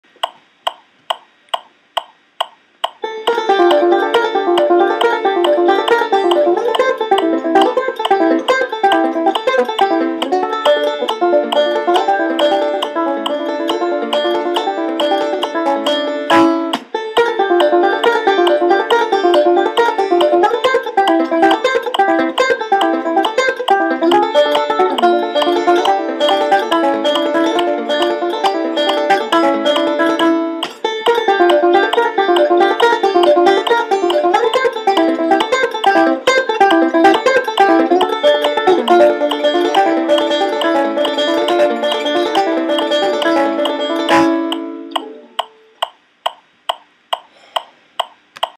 TornadoDrillClickTrack
Building speed on most instruments seems to work best to a metronome.  Here I've found a couple licks I like and am just drilling them over and over.